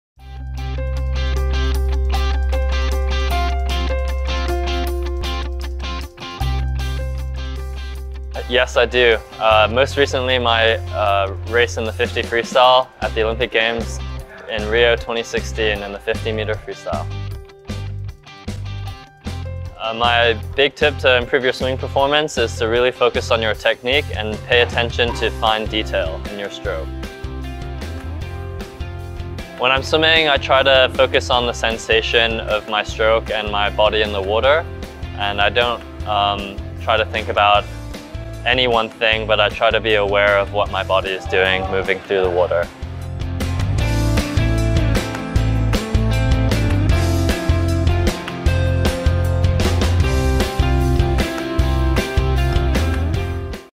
走近香港奧運游泳代表隊 之 謝旻樹篇
「男飛魚」謝旻樹 同大家分享佢提升游水表現嘅小秘訣！